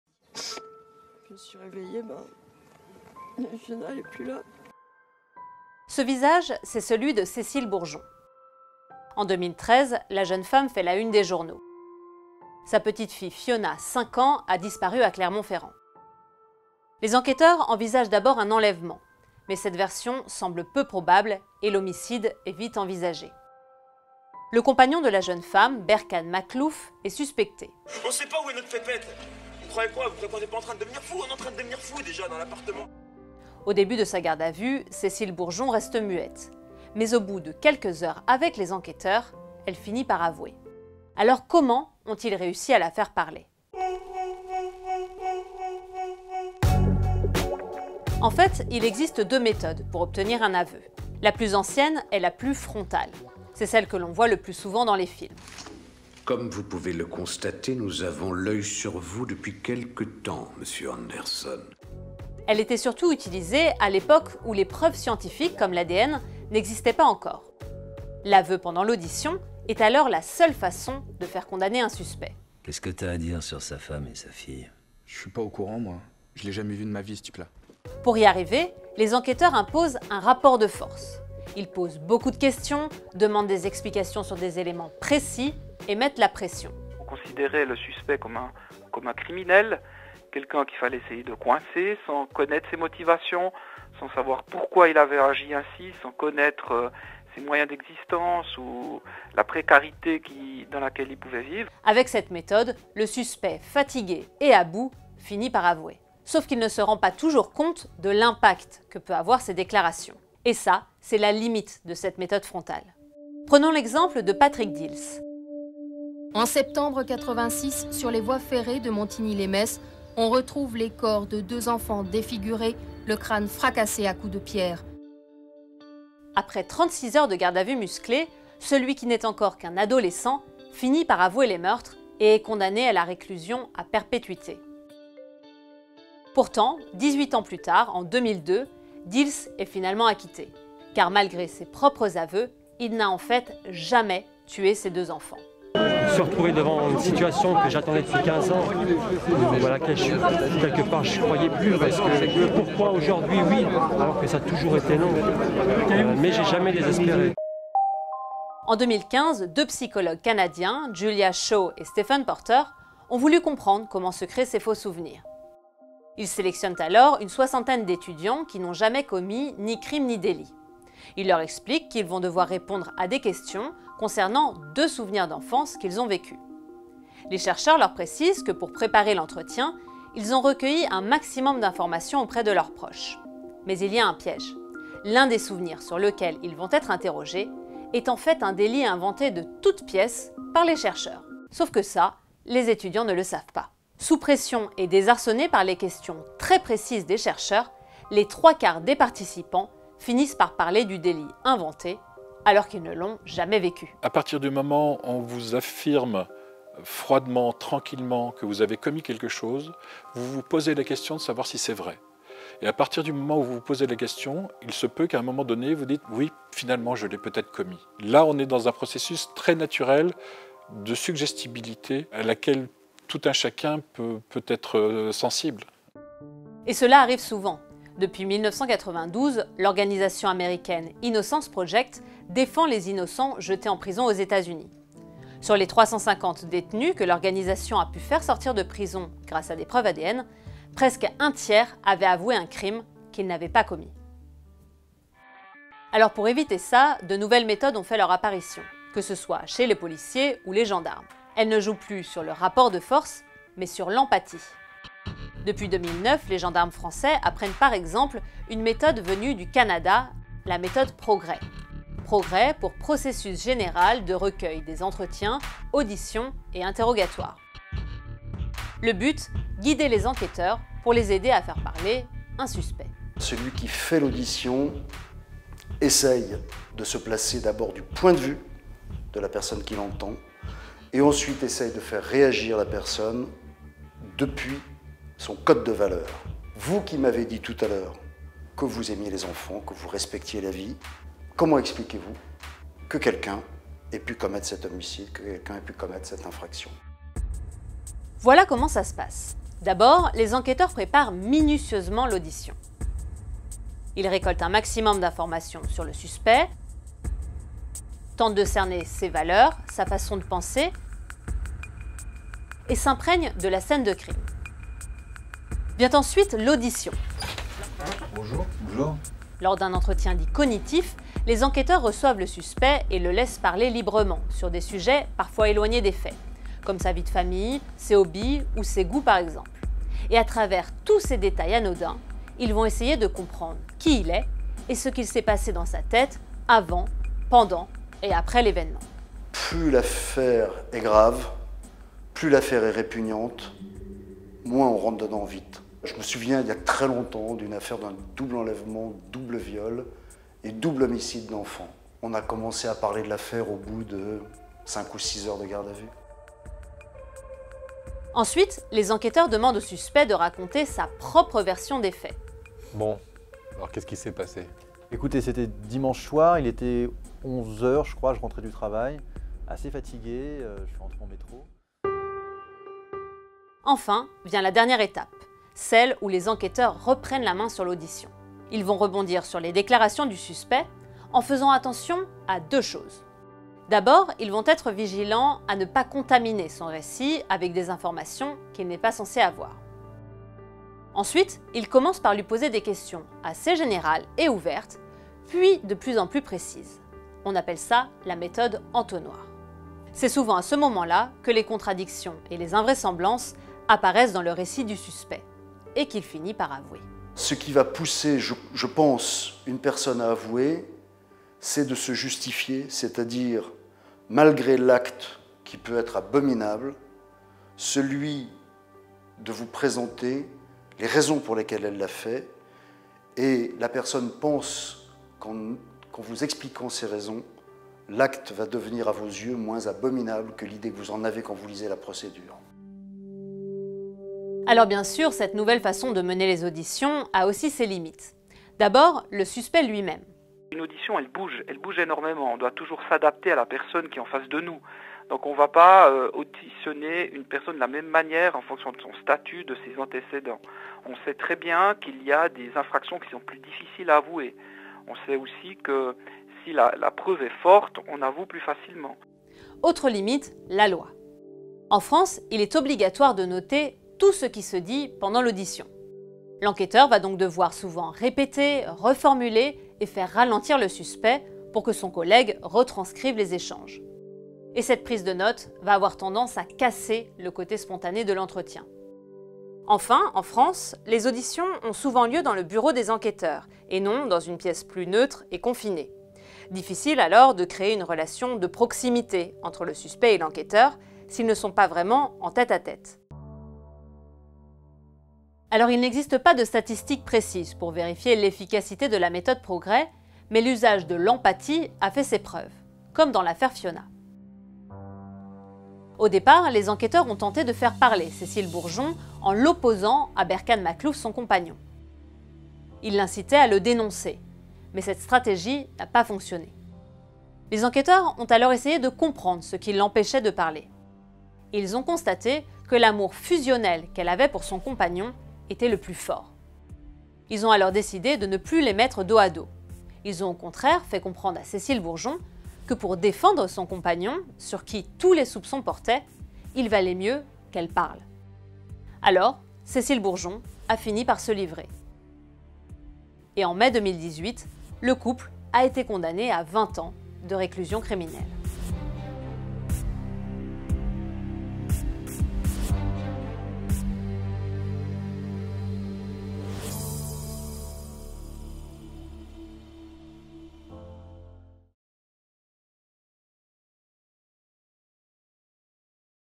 Comment les enquêteurs font-ils parler les suspects ?
Il y a ceux qui avouent de suite, ceux qui gardent le silence ou ceux qui mentent. Lors d’une audition, les enquêteurs font face à tous les profils de suspects. Comment font-ils pour les faire parler ? Avant l’arrivée des preuves ADN, l’aveu était essentiel pour faire condamner l’auteur d’un délit ou d’un crime. Pour y arriver, policiers et gendarmes utilisaient une méthode assez frontale basée sur le rapport de force. Pendant la garde à vue, ils mettaient la pression et posaient beaucoup de questions. Depuis quelques années, les méthodes ont évolué. L’empathie est désormais au coeur des auditions. Les enquêteurs essayent davantage de cerner le suspect pour comprendre ce qu’il avait dans la tête avant, pendant et après l’événement. Ils le laissent davantage parler et tentent de créer un climat de confiance. Chez les gendarmes, cette méthode porte un nom : la méthode Progreai.